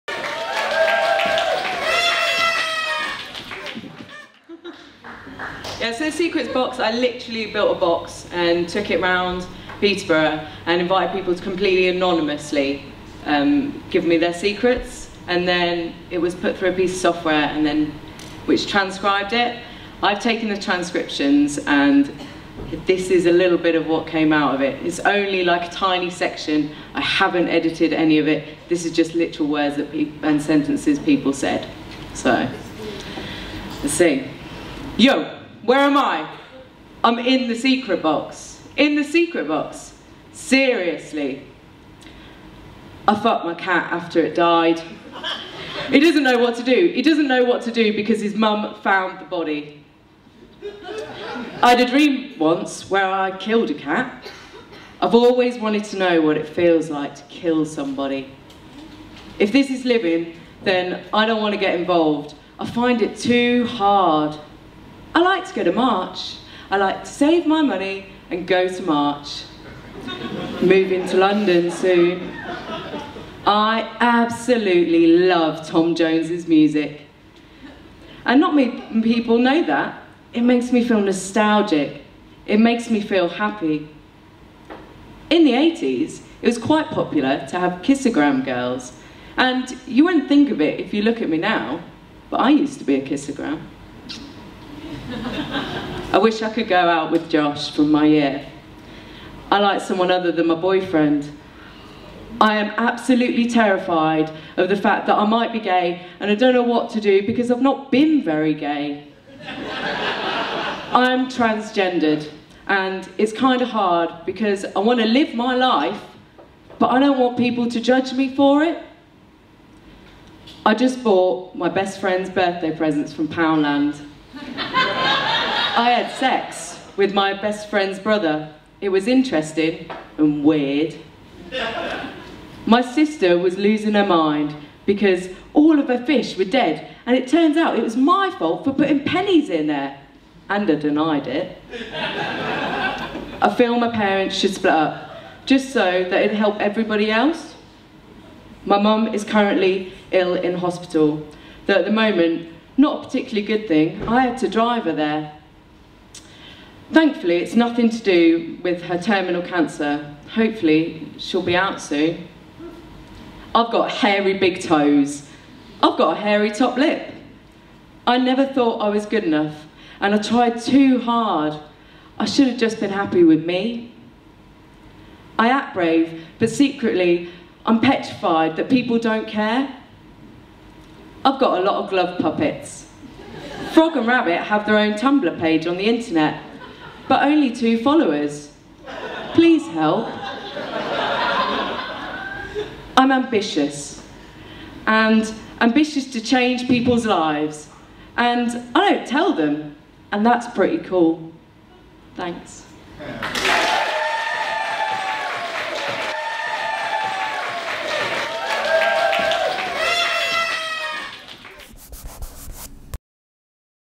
Lauren Kendrick at Freak Speak | RXCROSE
Lauren Kendrick at Freak Speak back in May 2016, Peterborough
This is part of a series of poets who featured at Freak Speak, Watch full Freak Speak film